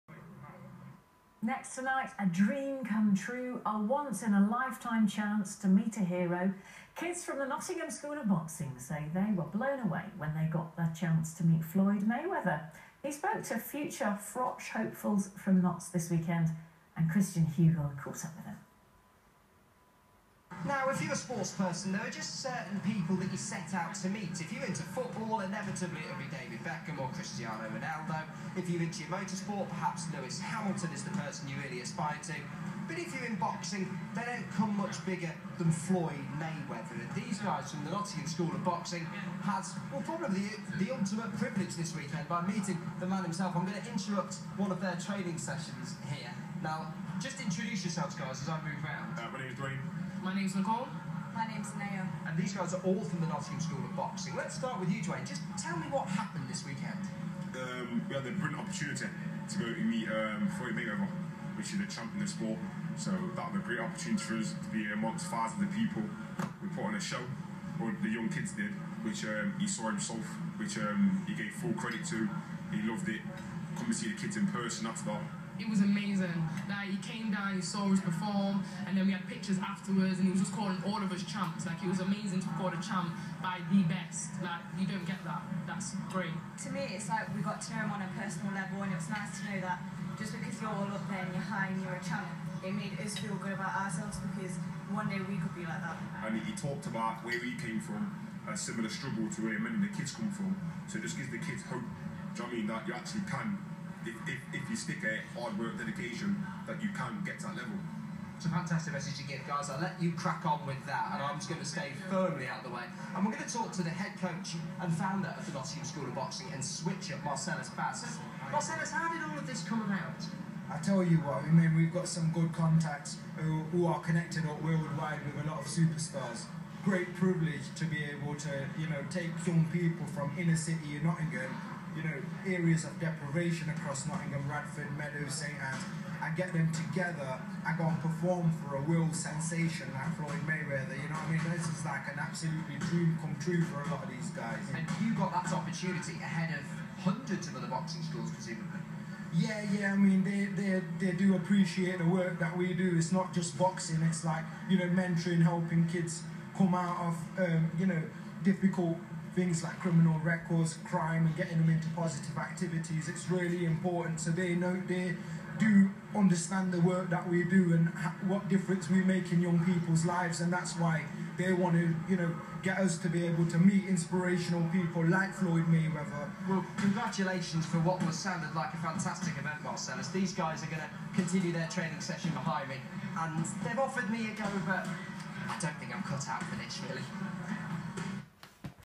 NOTTINGHAM SCHOOL OF BOXING YOUNG PEOPLE MEET WORLD SENSATION FLOYD MAYWEATHER!!
Nottingham school of Boxing/ Switch Up's young people perform for world sensation Floyd Mayweather.